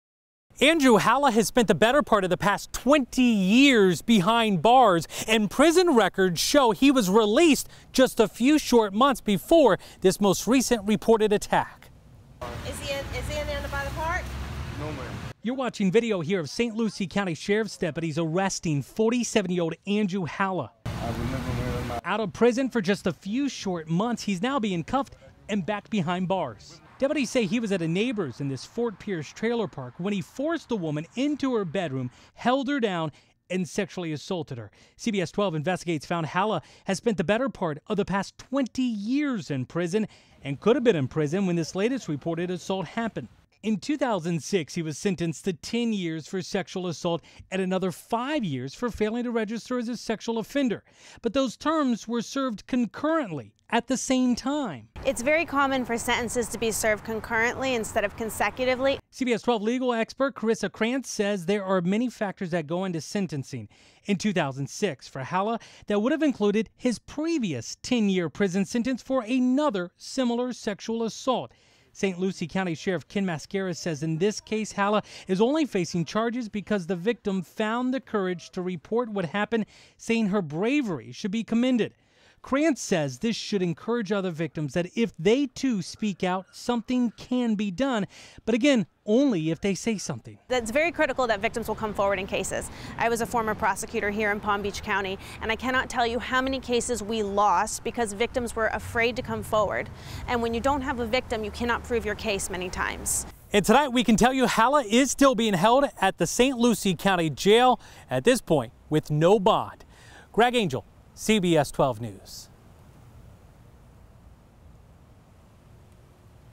CBS Carissa Kranz gives insight on the importance of victim testimony
Many cases are lost because victims are too intimidated to come forward to testify. Victims are to be commended for coming forward to hold someone accountable for their crimes.